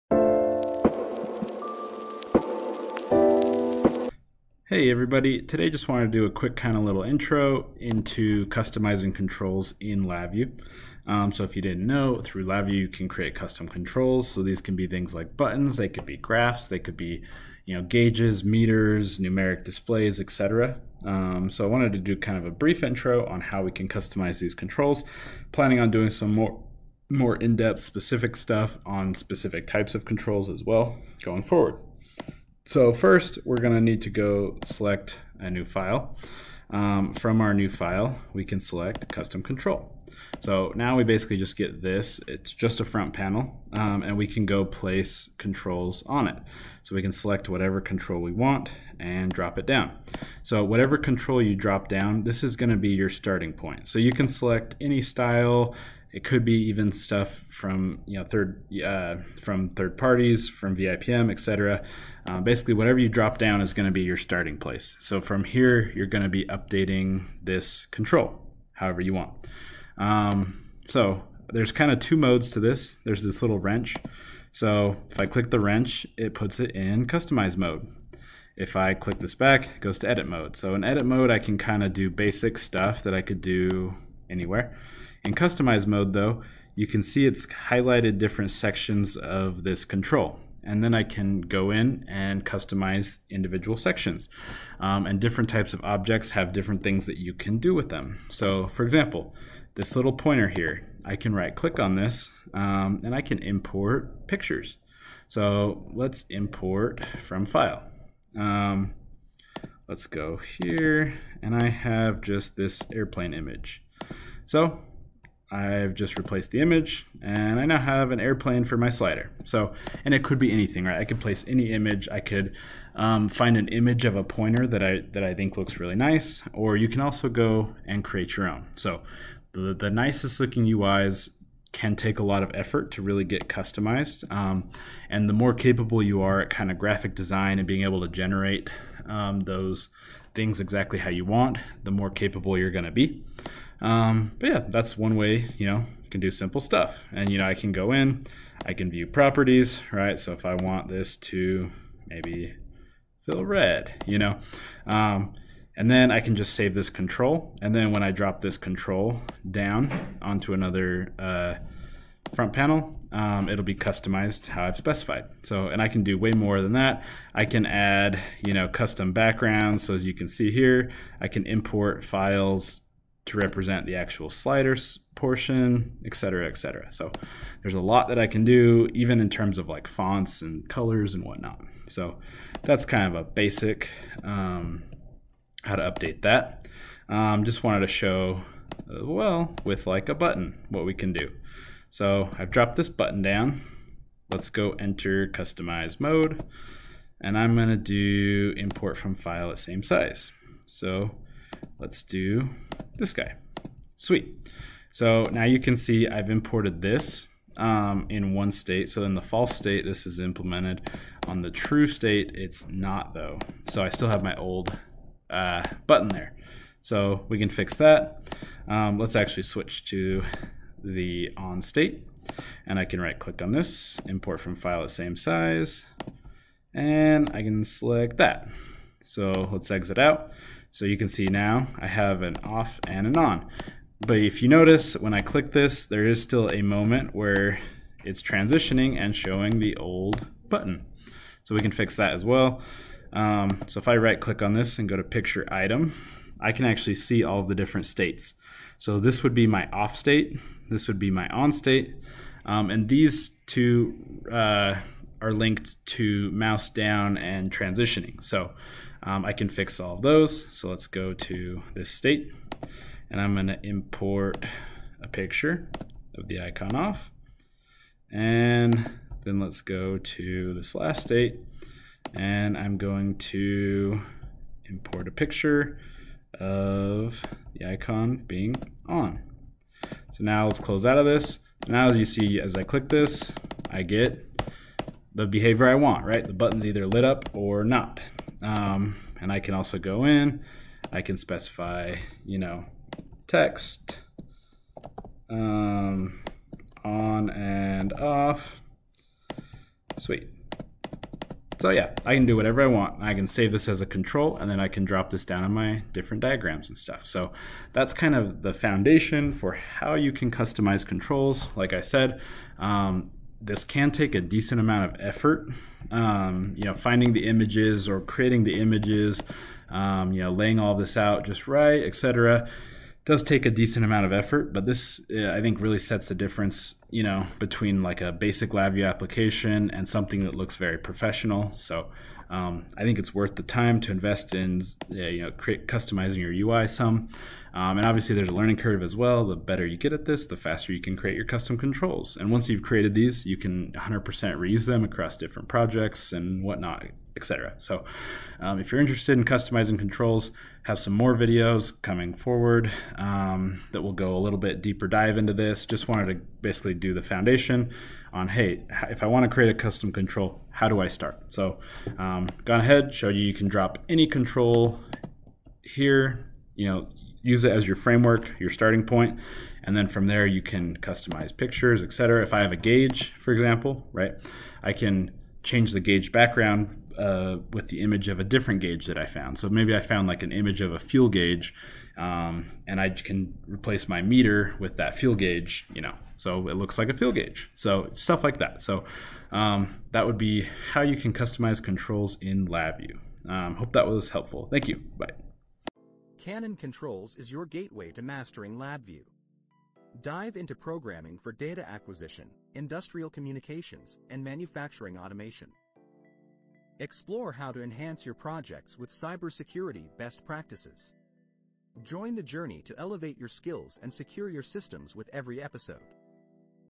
Customizing Controls in LabVIEW
In this video, learn how to customize controls in LabVIEW to design unique, professional, and intuitive user interfaces. Whether you want to enhance the visual appeal of your application or improve its functionality, customizing controls can take your LabVIEW projects to the next level.

What you’ll learn in this video:

-Introduction to Control Customization: Understand the importance of control customization for creating user-friendly interfaces.
-Customizing Built-in Controls: Learn how to modify existing controls by changing colors, sizes, and styles.
-Creating Custom Controls: Step-by-step guide to building your own controls using the Control Editor in LabVIEW.
-Enhancing Usability: Tips for improving interactivity and usability by tailoring controls to fit your application’s needs.
-Practical Examples: Watch real-world demos of customized controls in action, such as sliders, buttons, and indicators.

By the end of this video, you’ll have the skills to create visually appealing and functional controls that elevate your LabVIEW applications.